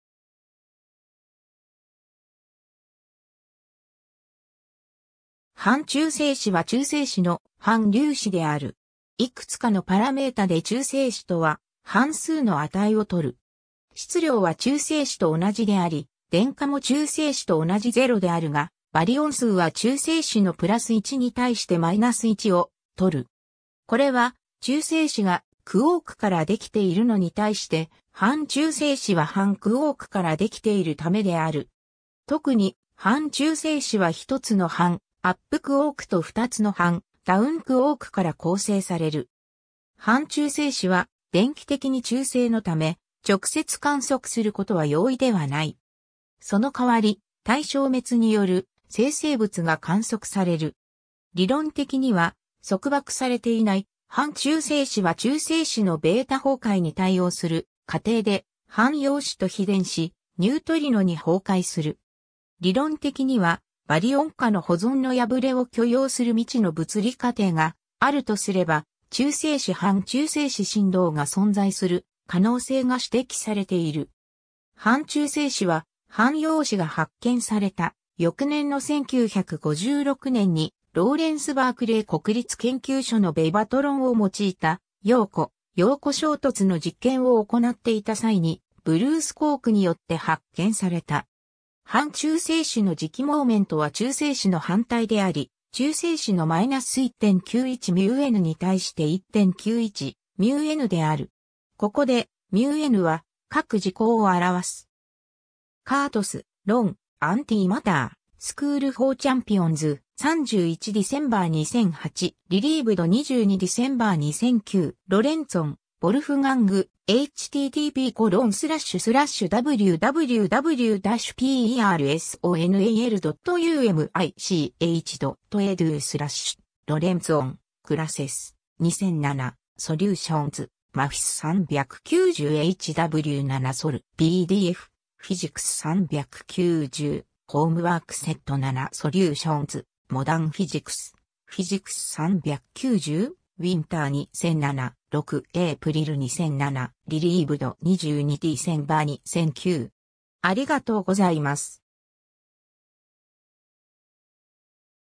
反中性子
反中性子（Antineutron）は中性子の反粒子である。
いくつかのパラメータで中性子とは反数の値を取る。
質量は中性子と同じであり、電荷も中性子と同じゼロであるが、バリオン数は中性子の+1に対して-1を取る。
これは、中性子がクォークから出来ているのに対して、反中性子は反クォークから出来ているためである。
特に、反中性子は1つの反アップクォークと2つの反ダウンクォークから構成される。
 反中性子は電気的に中性のため、直接観測することは容易ではない。
その代わり、対消滅による生成物が観測される。
理論的には、束縛されていない反中性子は中性子のベータ崩壊に対応する過程で、反陽子と陽電子、ニュートリノに崩壊する。
理論的には、バリオン価の保存の破れを許容する未知の物理過程があるとすれば、中性子-反中性子振動が存在する可能性が指摘されている。
 反中性子は、反陽子が発見された翌年の1956年に、ローレンス・バークレー国立研究所のベバトロンを用いた陽子-陽子衝突の実験を行っていた際に、ブルース・コークによって発見された。
 反中性子の磁気モーメントは中性子の反対であり、中性子の-1.91 μNに対して1.91 μNである。
ここで、μNは核磁子を表す。
^ Kurtus, Ron: "Antimatter", "School for Champions.",31 December 2008. Retrieved 22 December 2009. ^ Lorenzon, Wolfgang "Physics 390: Homework set #7 Solutions", "Modern Physics, Physics 390, Winter 2007." 6 April 2007. Retrieved 22 December 2009.